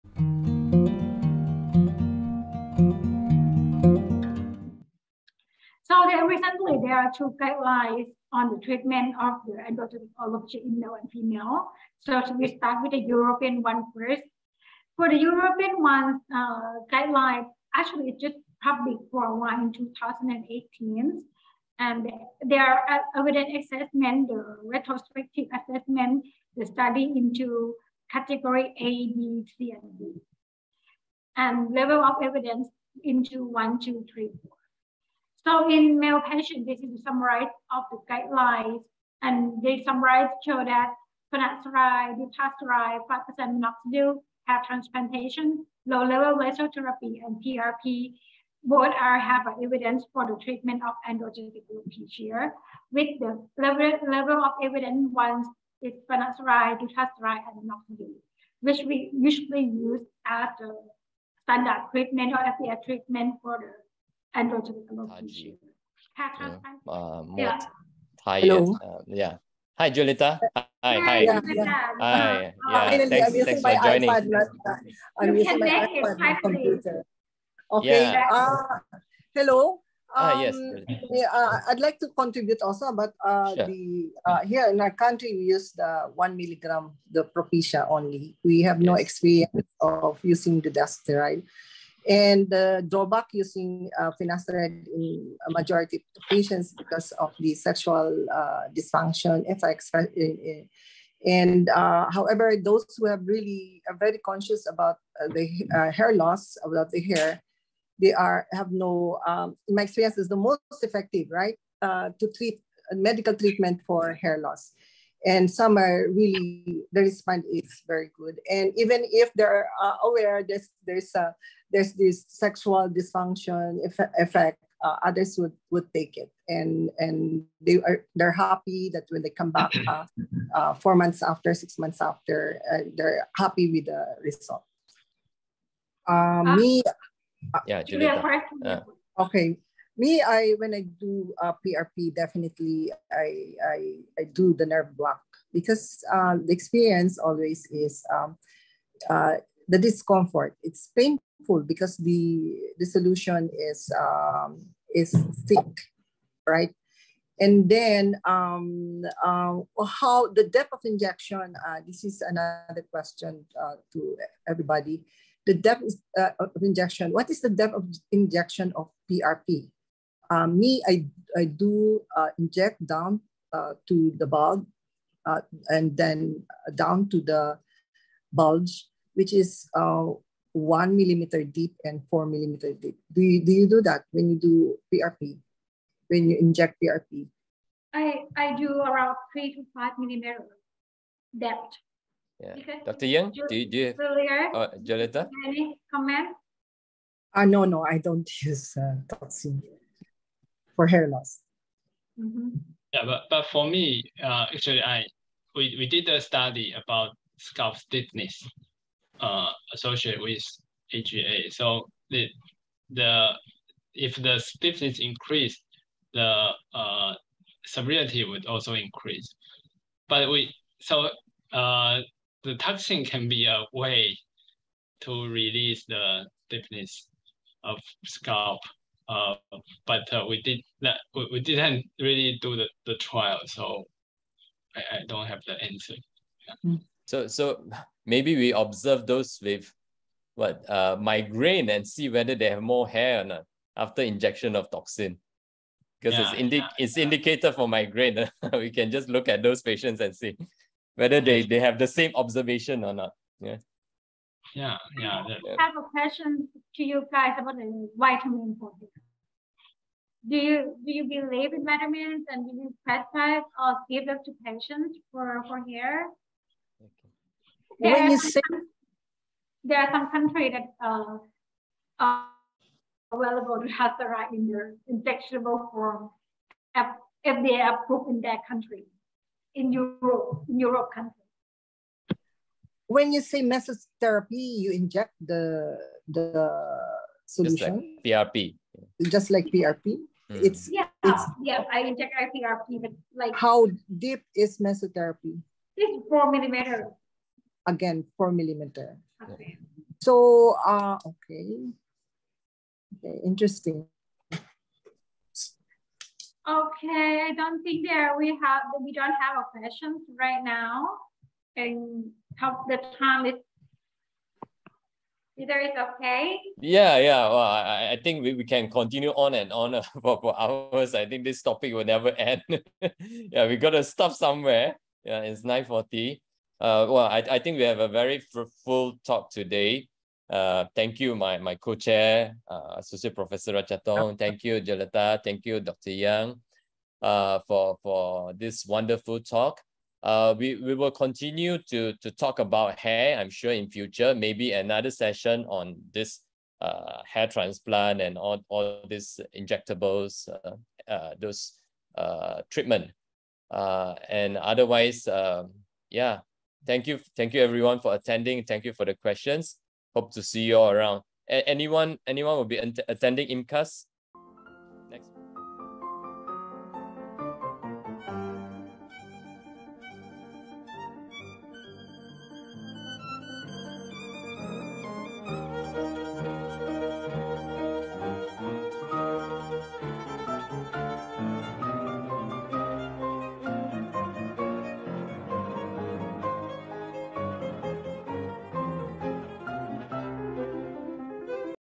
[Teaser] Panel Discussion: How Expert Approach and Prescribe Prescription for Male and Female Pa...
Clinical Dermatology and Regenerative Medicine

Asso. Prof. Dr. Ratchathorn PANCHAPRATEEP
Associated Professor, Division of Dermatology, Chulalongkorn University, Bangkok, THAILAND

Prof. Dr. Chao-Chun YANG
Professor and Chairman, Department of Dermatology, National Cheng Kung University Hospital, Tainan, TAIWAN

Dr. Julieta Peralta ARAMBULO
Head, Hair Transplant Service at the Aesthetic Wellness Center of The Medical City, Pasig, Metro Manila, PHILIPPINES